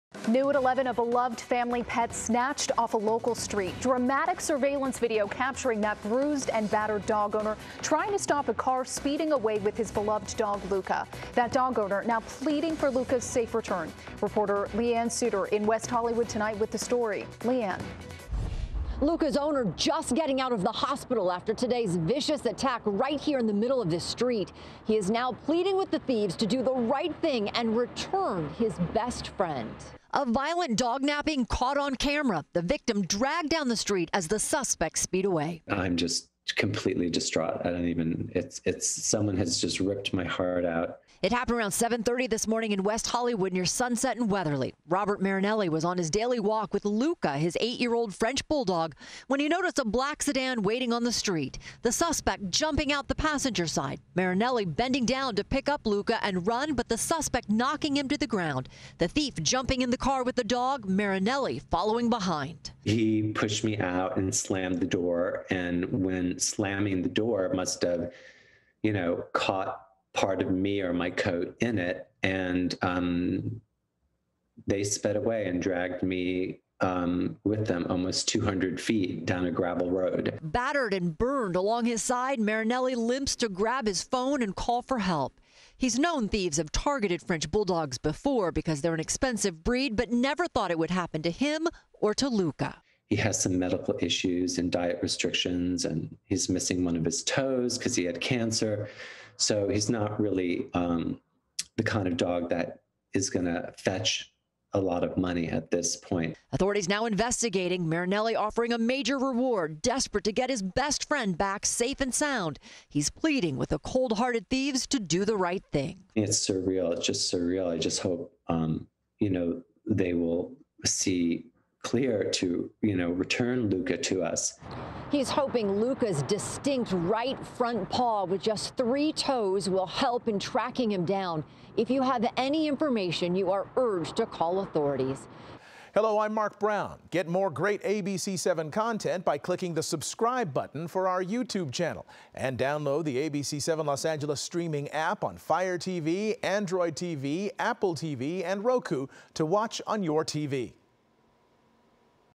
Video: French bulldog stolen during brutal attack on owner in West Hollywood l ABC7
Shocking video shows the moment a man was dragged nearly 200 feet after his clothing got caught in the car as he was trying to save his French bulldog from being stolen in West Hollywood. MORE